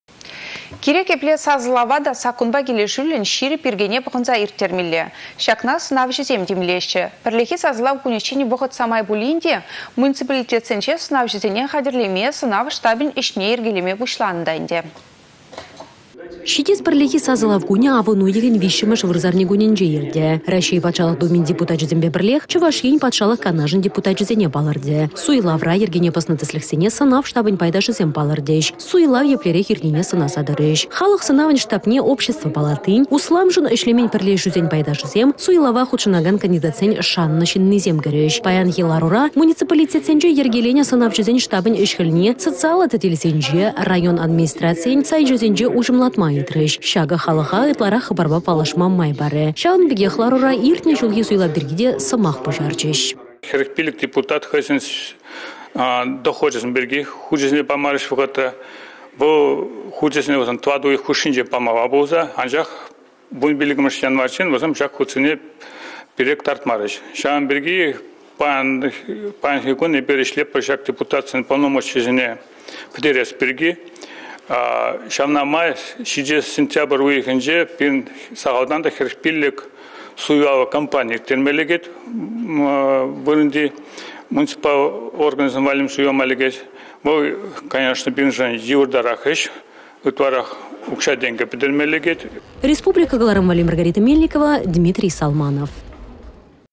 Халăх сăнавĕн штабĕсем йĕркеленеççĕ
Кирек епле сасăлăва та саккунпа килĕшÿллĕн, çирĕп йĕркене пăхăнса ирттермелле. Çакна сăнавçăсем тимлеççĕ. Пĕрлехи сасăлав кунĕччен вăхăт самай пулин те, муниципалитетсенче сăнавçăсене хатĕрлеме, сăнав штабĕн ĕçне йĕркелеме пуçланă та ĕнтĕ.